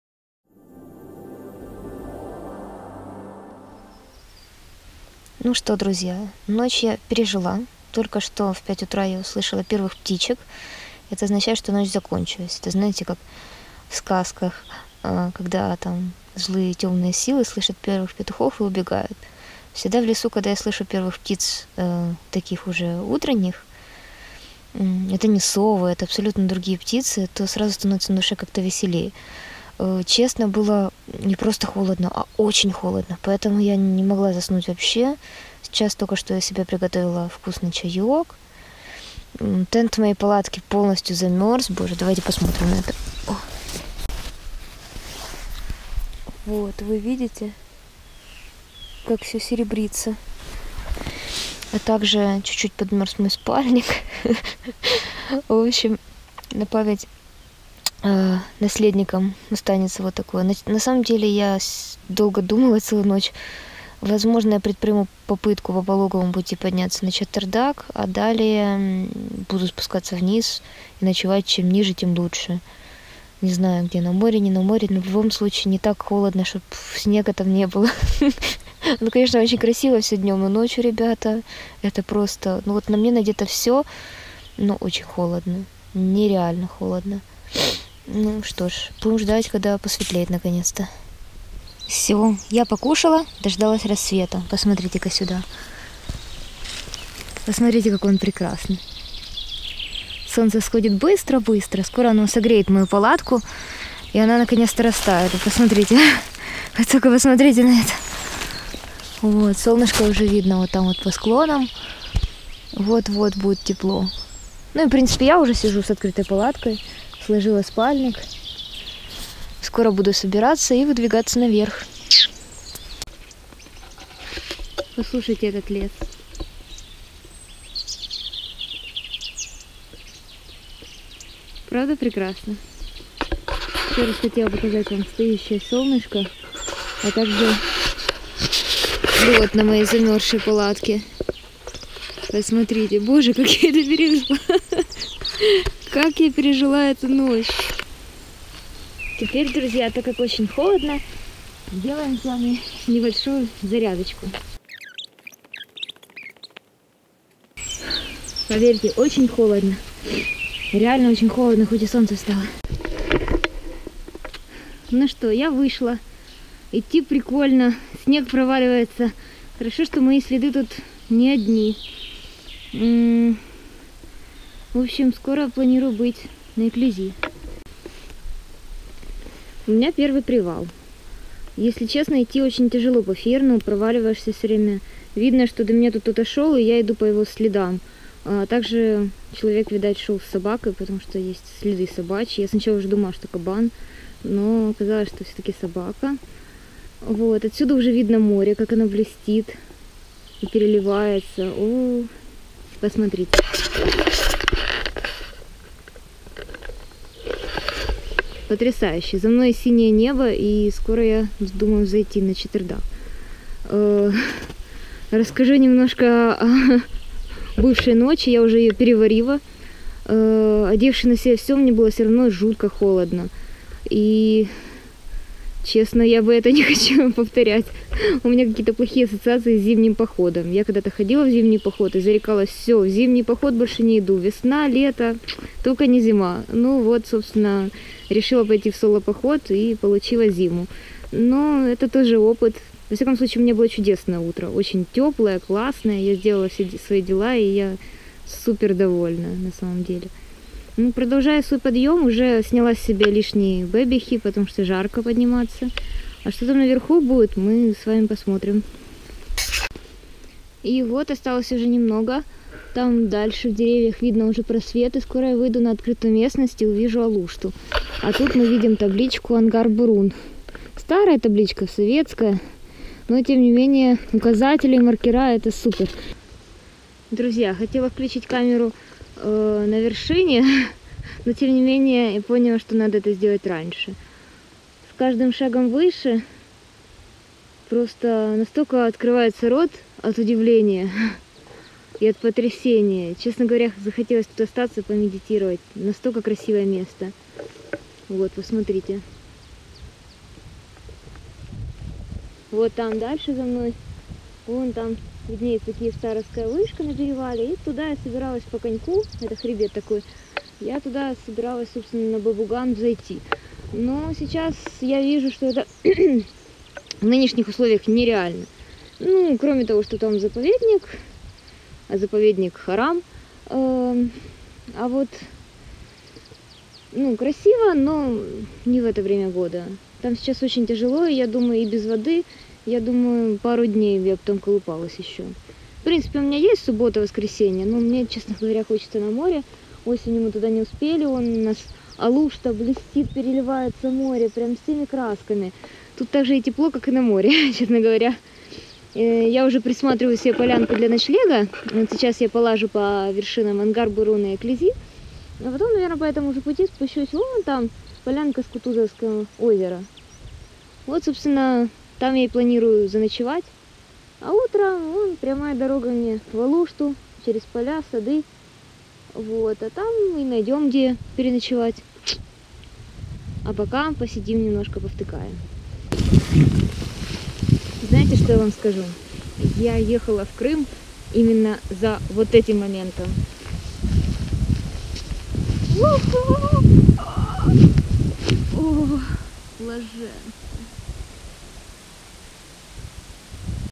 Alone in the mountains. Vlog Part 3. The way to Chatyrdag
My first solo travelling to Crimea mountains. Crossing the border, living in the camp high in the mountains, visiting Chatyr Dag and Alushta bay